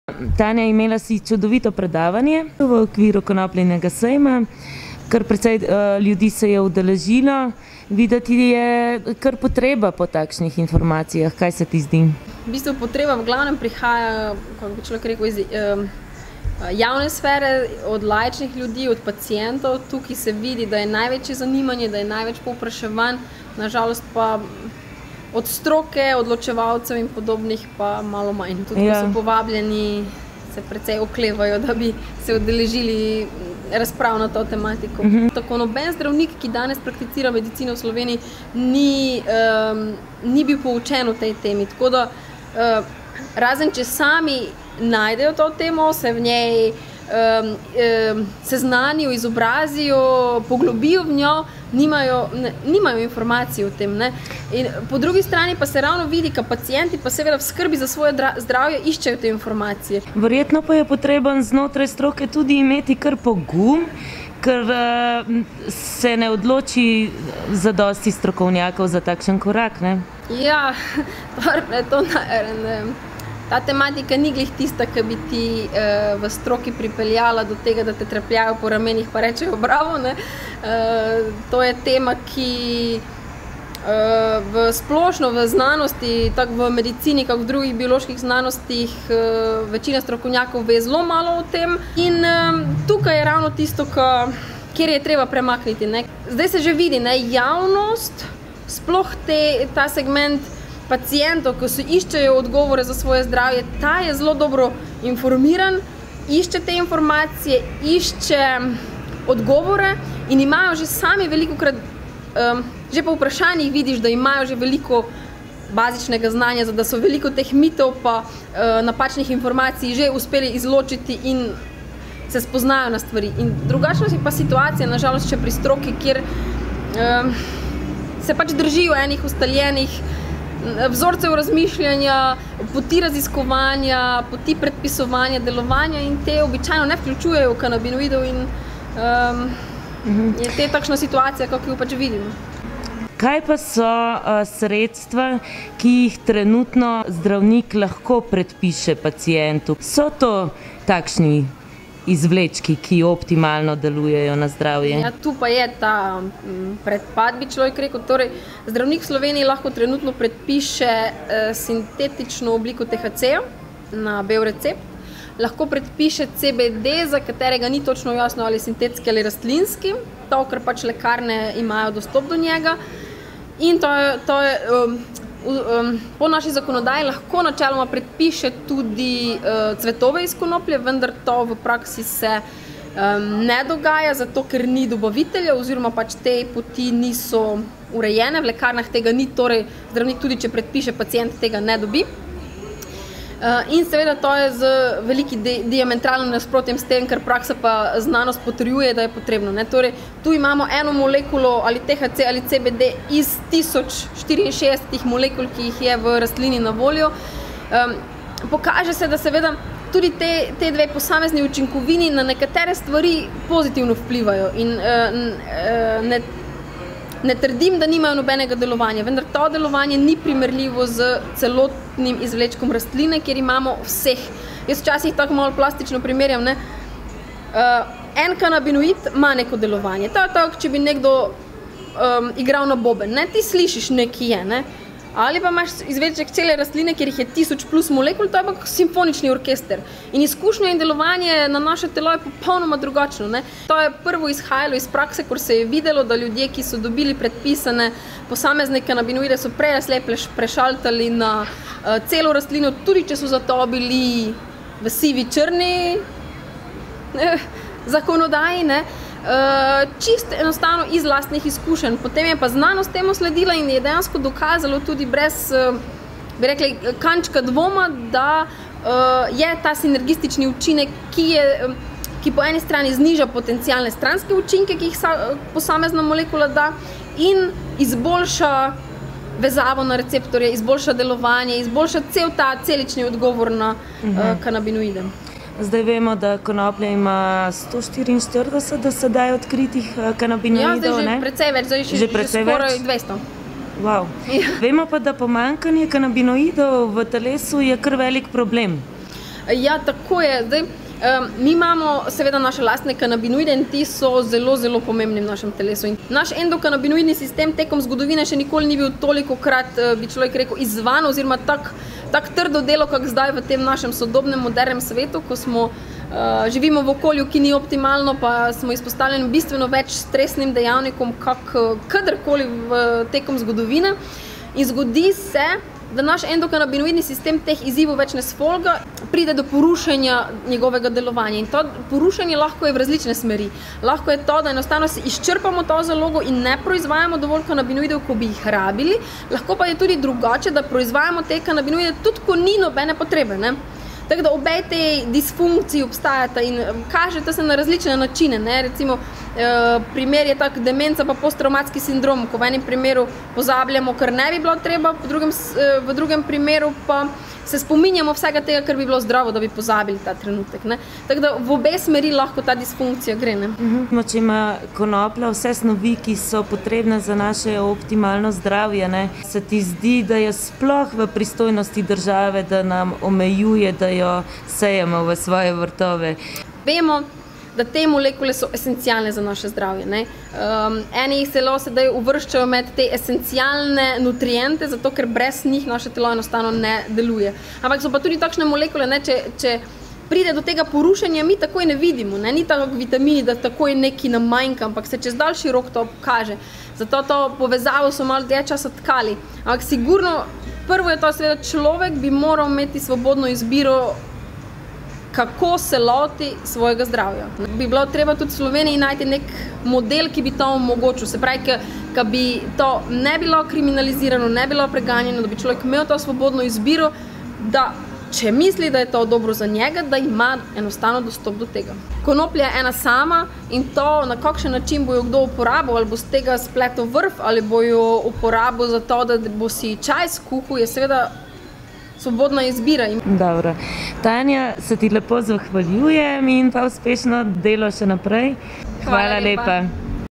Dr. Tanja Bagar: Konoplja je ena sama
Se v strokovnih krogih odnos do konoplje spreminja?  Bi svoj proces zdravljenja s konopljo zaupali nekomu, ki nima izkušenj in tudi ne znanja o konoplji?  Kako učinkovita konopljina sredstva lahko dobimo preko recepta?
Dr. Tanja Bagar je predavala v sklopu konopljinega sejma, organizator dogodka: konoplja.net, drštvo PEST